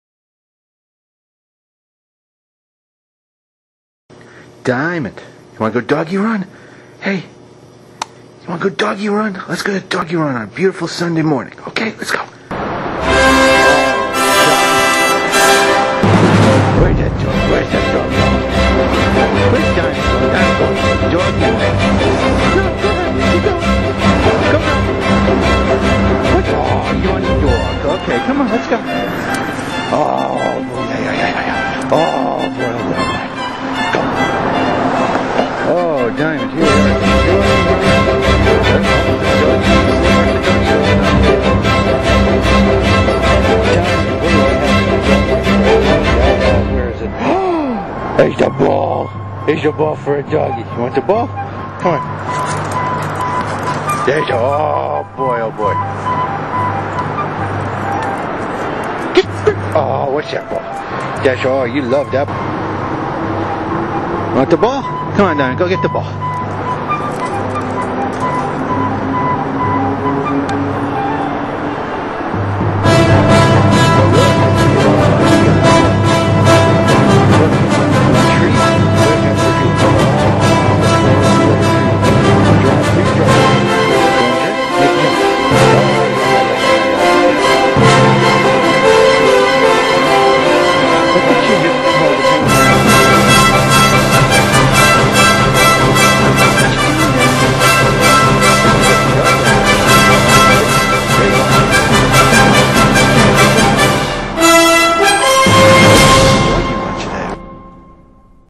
DiamondDogRun
Diamond at the dog run, 7 May 2006.